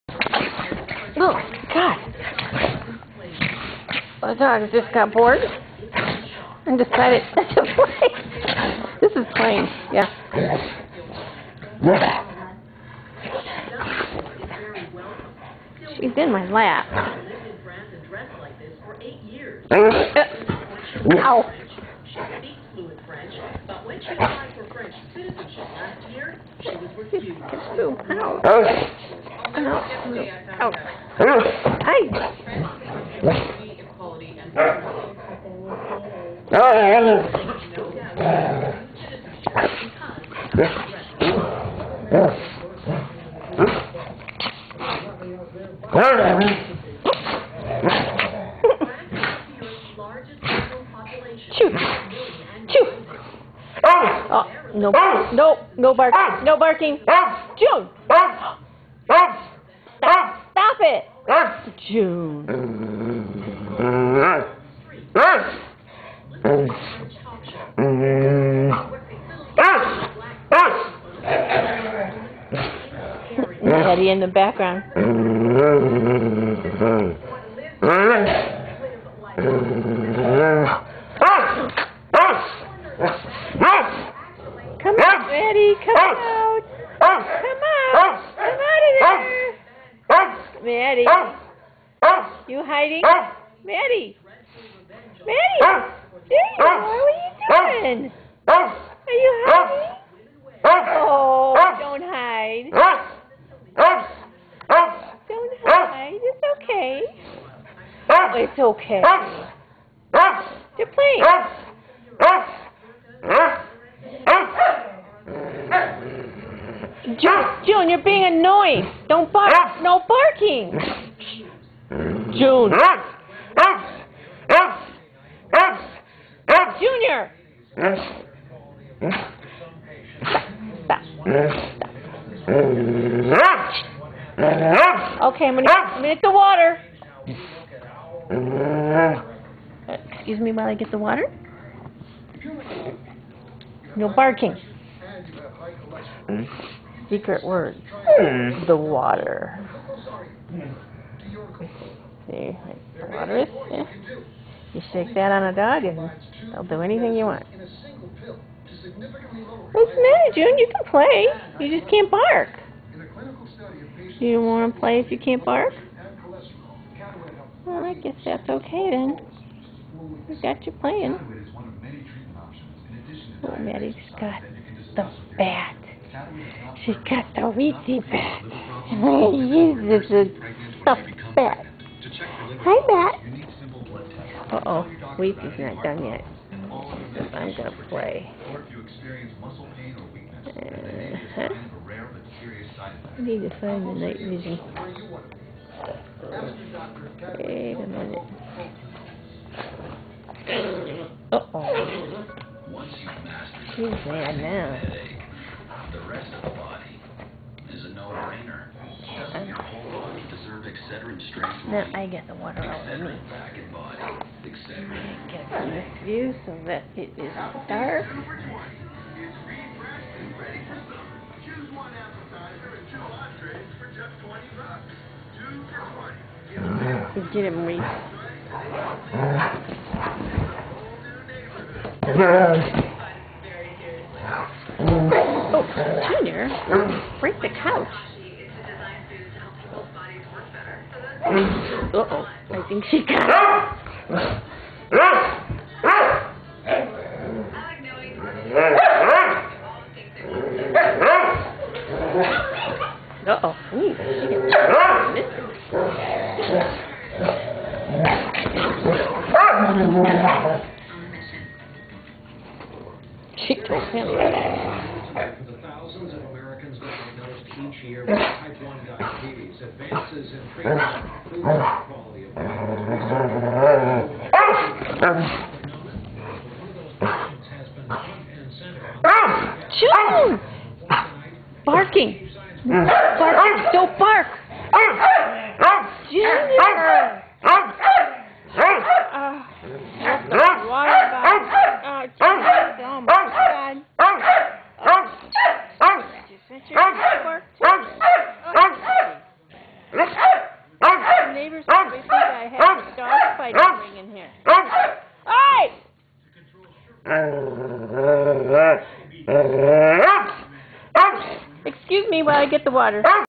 Weetzie Starts A Ruckus
Weetzie was bored so she harassed Junior until he started to play with her. Maddy protested the whole thing.